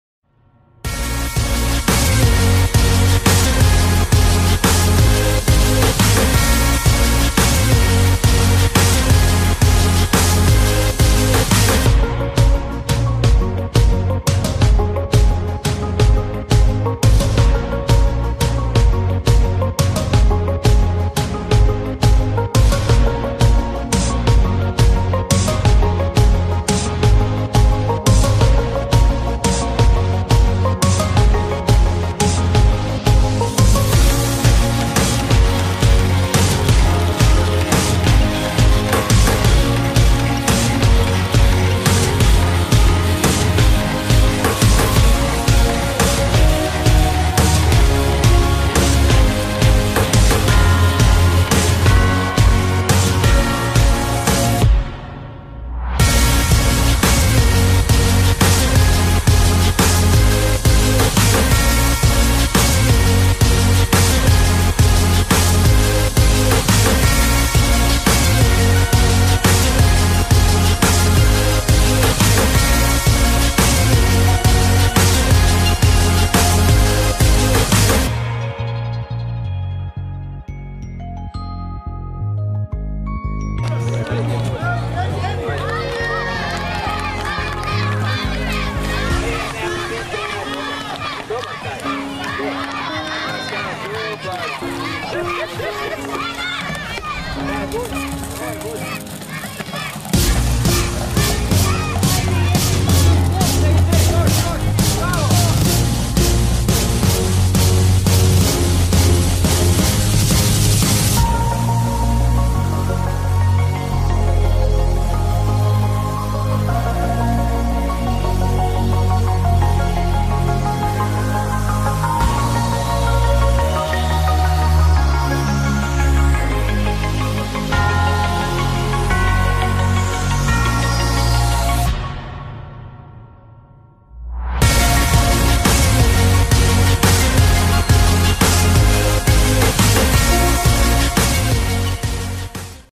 16. kup HVZ-a "Antun Brlaković", Popovača, DVD Nove plavnice i kž.cesta HD
Nastup ekipa mladeži i djece DVD-a Nove plavnice i križevačka cesta na 16. kupu HVZ-a  "Antun Brlaković" u Popovači, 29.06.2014.

photo & video
by tomislay

Using it just for background.